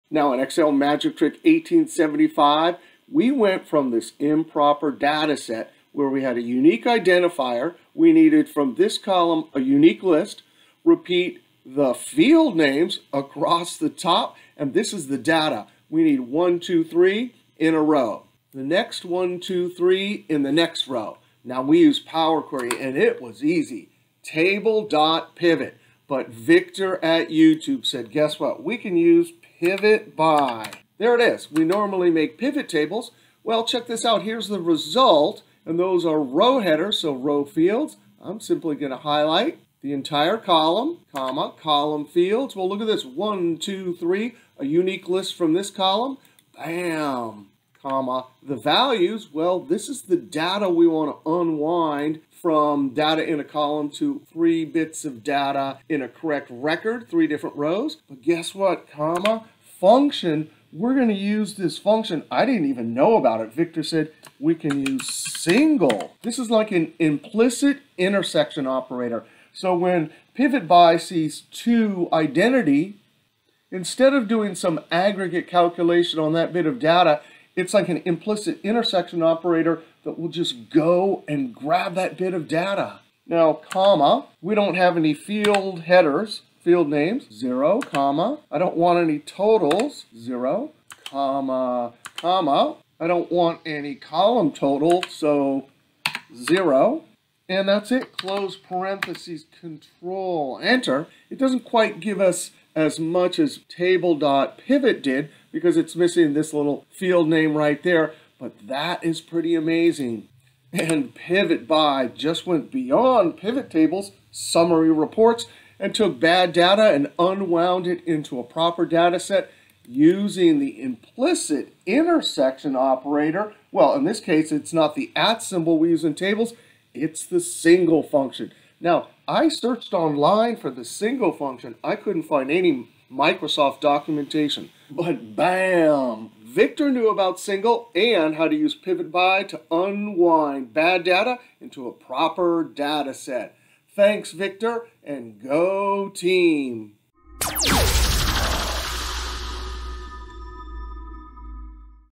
Unbelievable PIVOTBY & SINGLE Functions to Covert Bad Data Into Proper Table. Excel Magic Trick 1876
Learn how to convert to bad data table with records of data in columns to a proper table structure with records in rows using the PIVOTBY & SINGLE Functions. This trick comes from viktorasgolubevas at YOUTUBE.
Topics:
1. (00:00) Introduction.
2. (00:25) PIVOTBY & SINGLE Functions Solution.
3. (01:48) Summary
4. (02:23) Closing